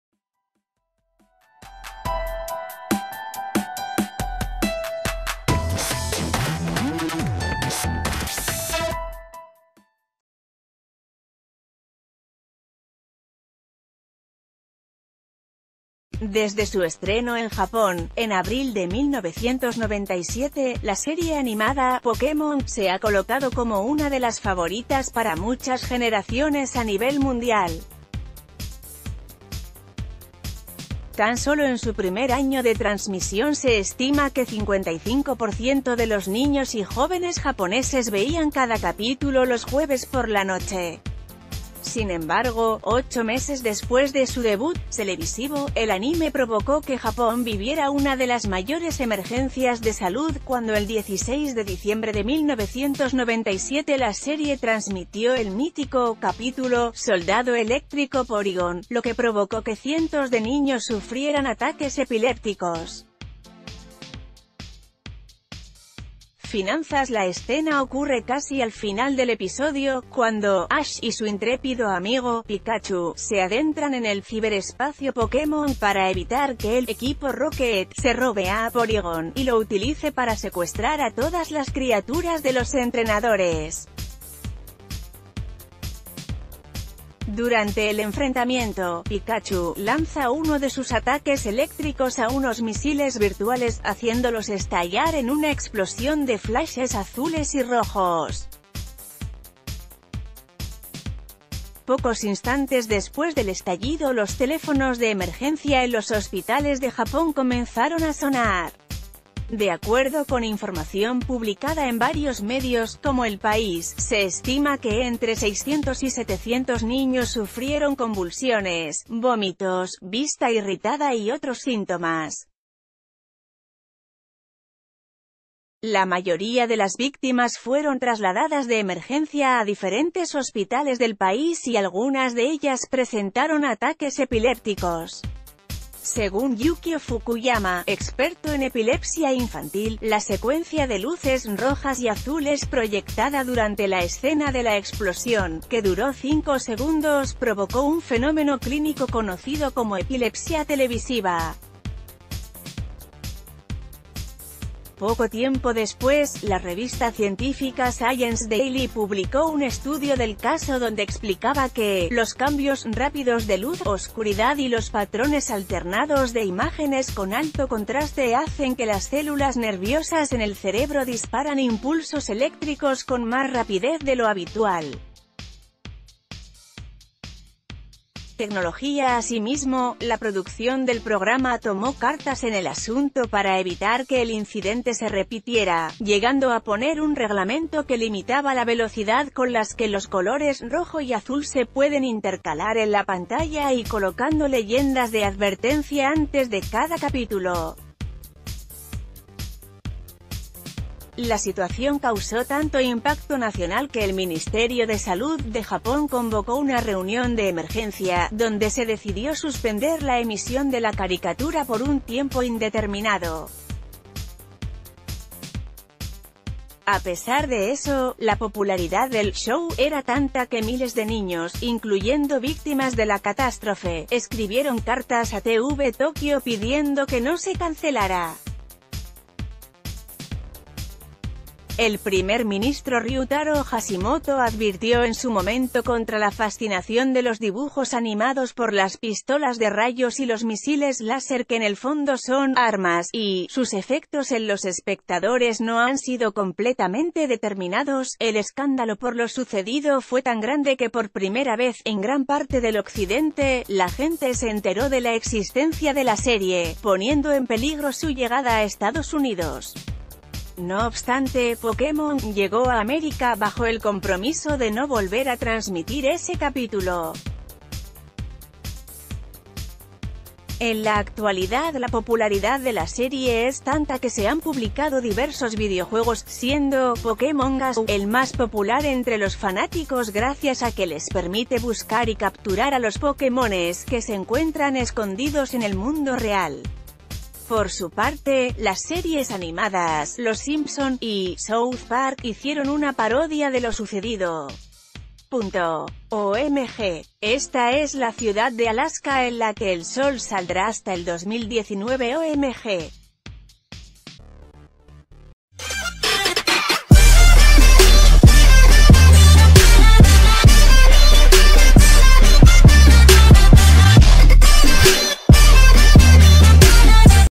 Pokémon cumple 21 años de su estreno mundial
Desde su estreno en Japón, en abril de 1997, la serie animada “Pokémon” se ha colocado como una de las favoritas para muchas generaciones a nivel mundial. Tan sólo en su primer año de transmisión se estima que 55 por ciento de los niños y jóvenes japoneses veían cada capítulo los jueves por la noche...